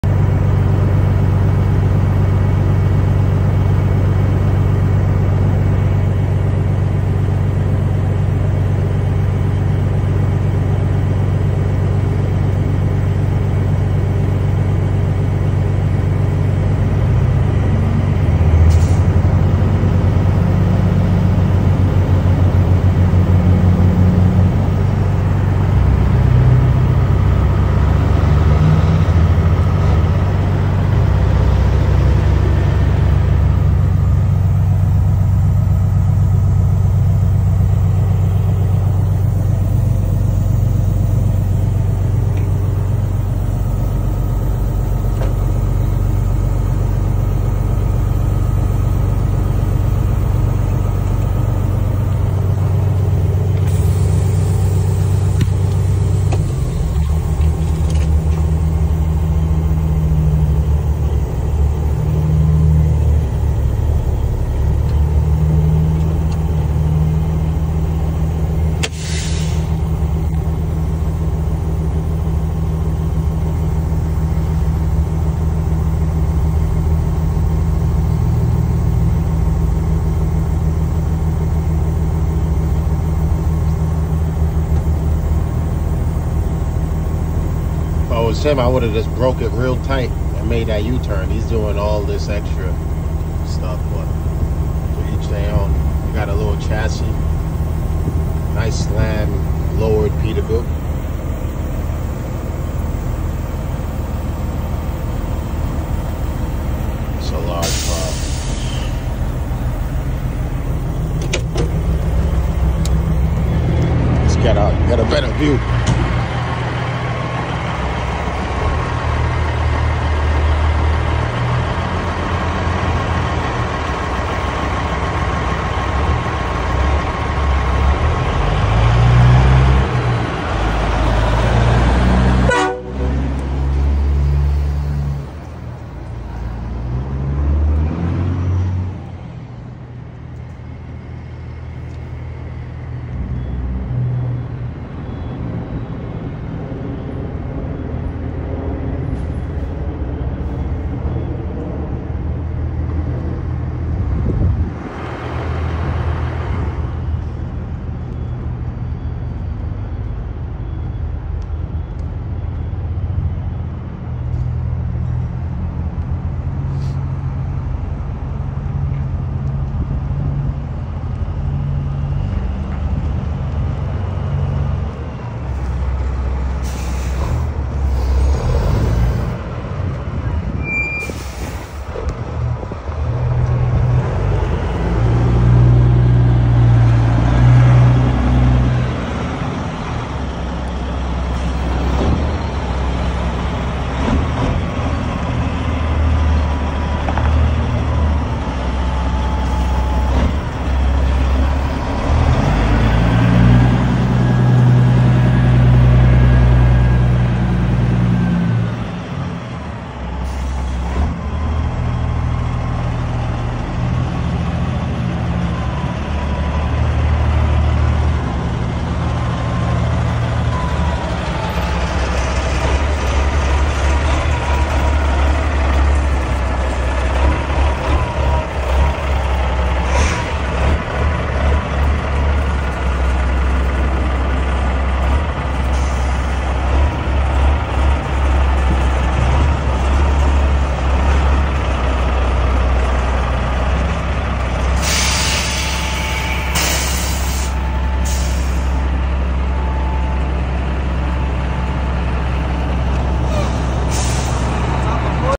Custom working class 379 peterbilt in NYC
310 wheelbase peterbilt docking for amazon